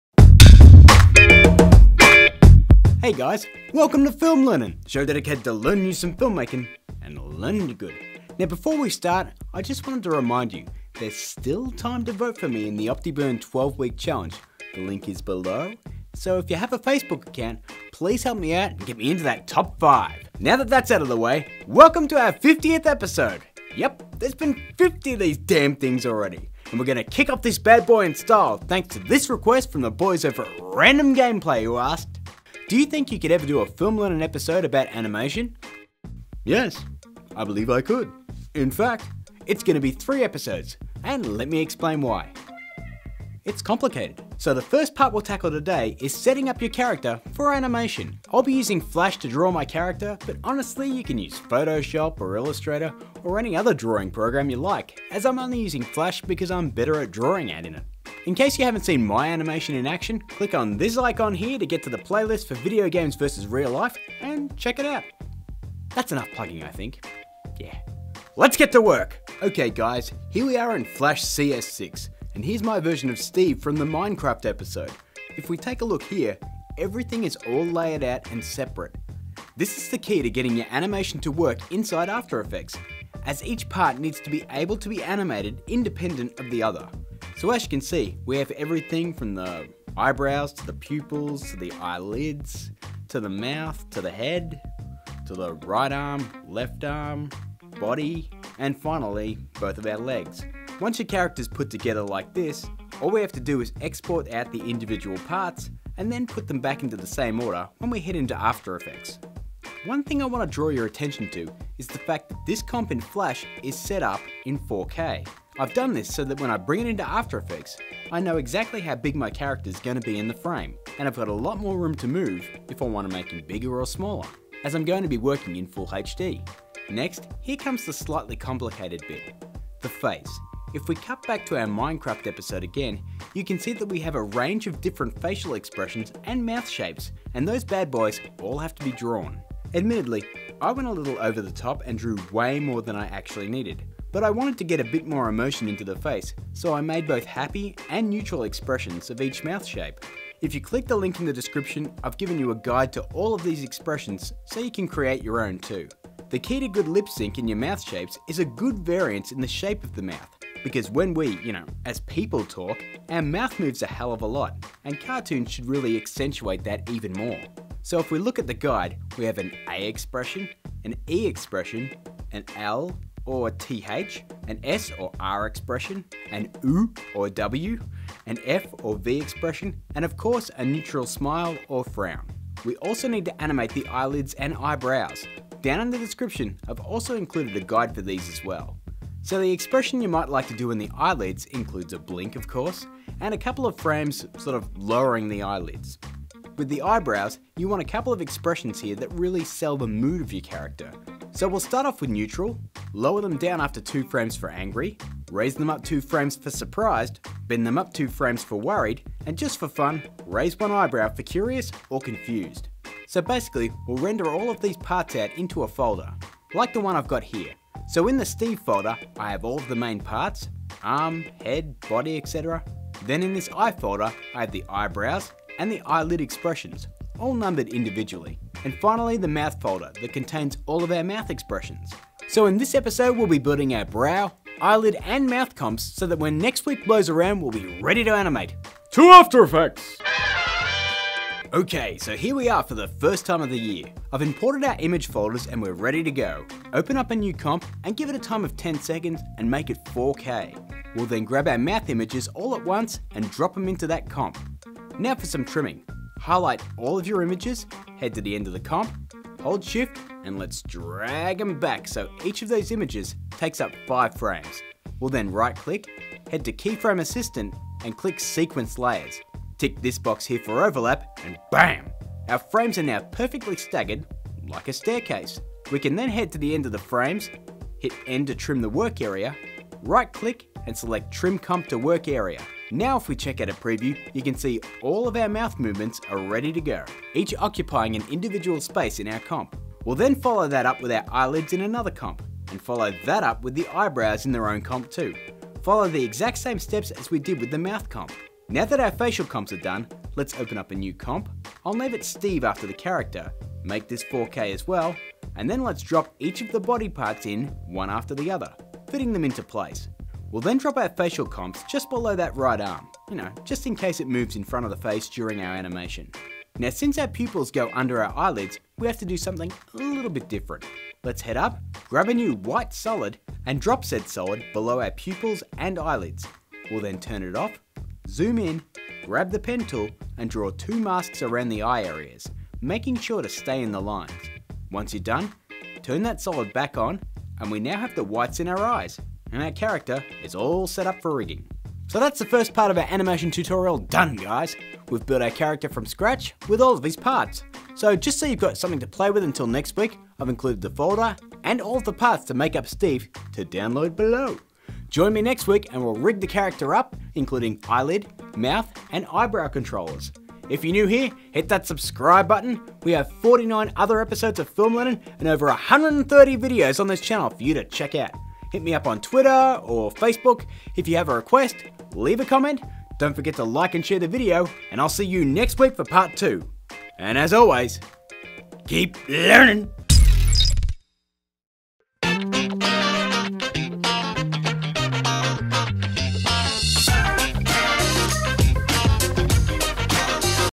Film Learnin: Basic Animation Tutorial - Part 1
It's our 50th episode! We're kicking off 2015 with the first of our 3 part series on animating in After Effects!

20% Annual subscriptions code: MAHGAWD_ANN